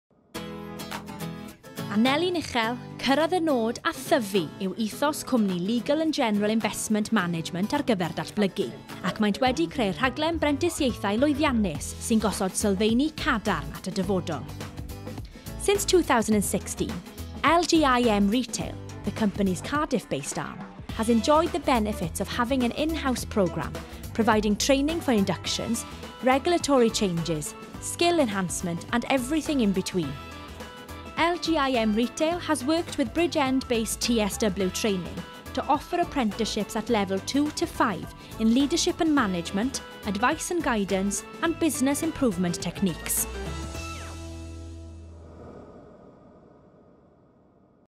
Legal and General
Apprenticeship Awards Cymru 2019 - Large Employer of the Year
Gwobrau Prentisiaethau Cymru 2019 - Cyflogwr Mawr y Flwyddyn